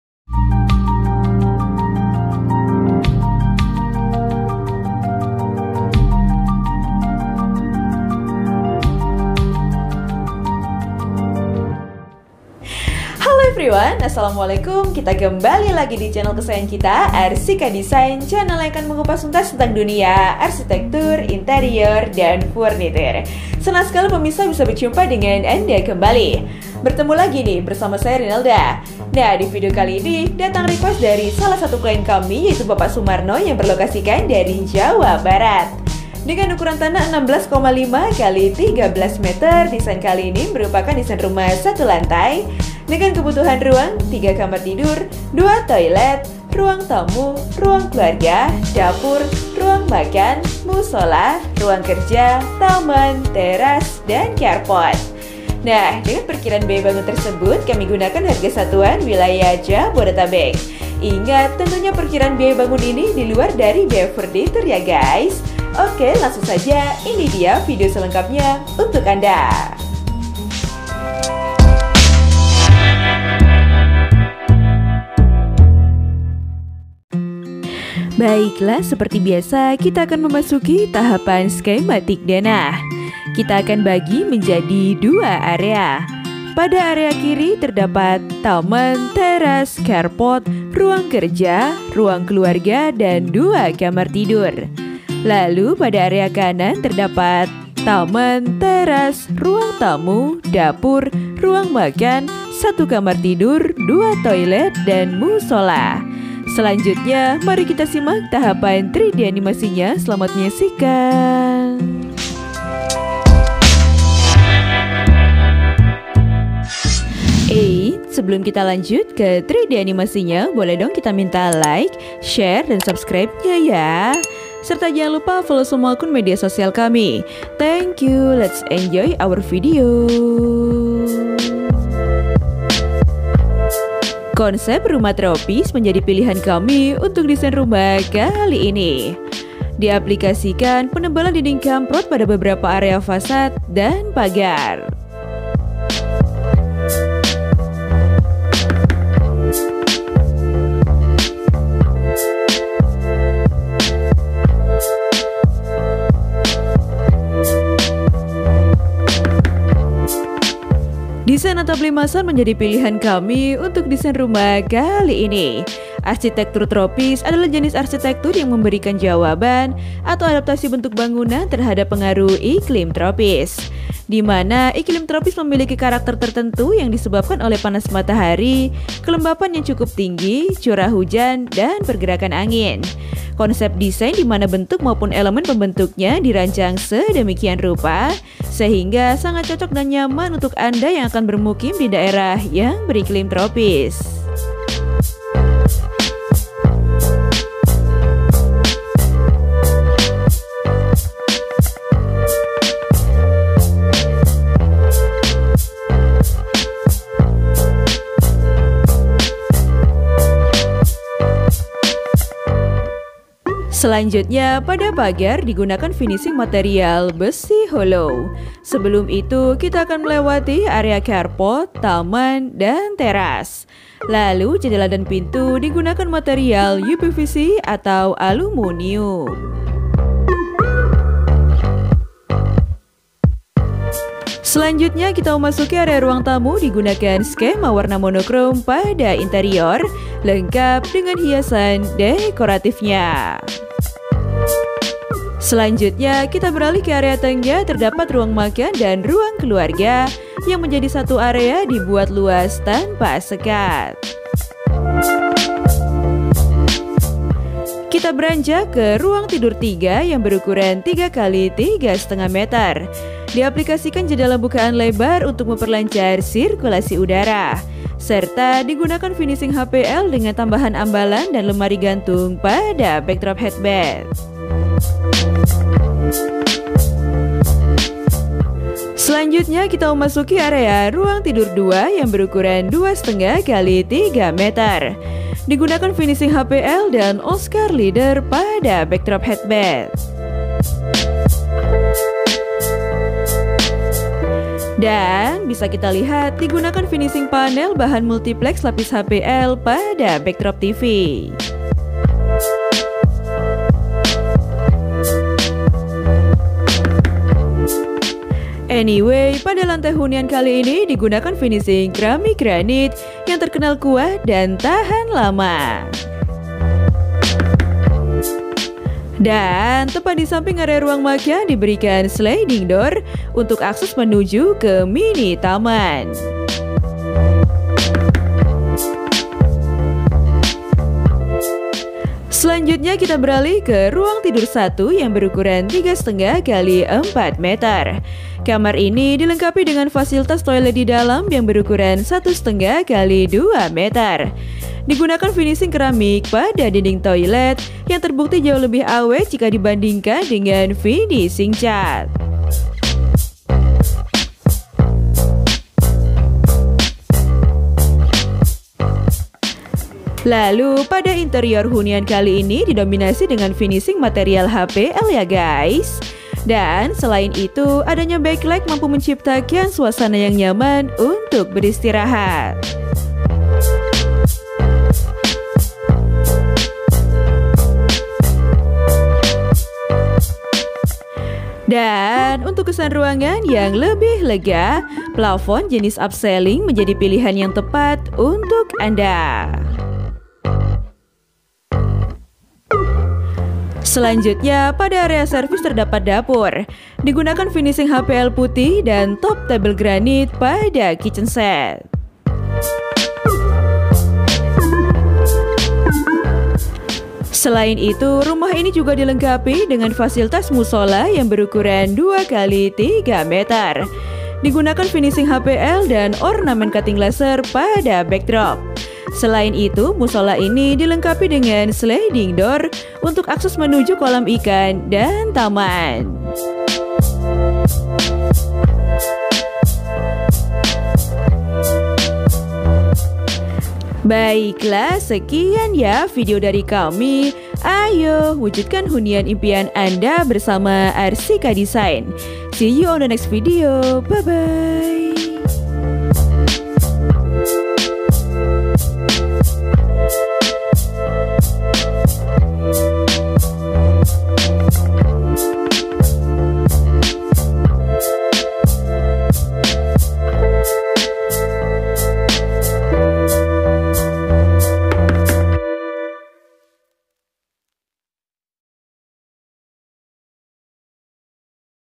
DESAIN RUMAH 16.5 x 13 meter I Jawa Barat – DESAIN RUMAH TROPIS
Video kali ini membahas desain rumah request dari klien kami dengan Bapak Sumarno  yang berlokasikan di Jabar,  dengan informasi sbb :
Ukuran Tanah = 16.5 x 13 meter
Luas Bangunan =   120  meter2

Perkiraan Biaya bangun =  600 Juta – 700 Juta  (Wilayah Jabodetabek – diluar biaya furnitur)

KEBUTUHAN RUANG :
- 3 Kamar Tidur
- 2 Toilet
- Ruang Tamu
- Ruang Keluarga
- Dapur
- Ruang Makan
- Mushola
- Ruang Kerja
- Taman
- Teras
- Carport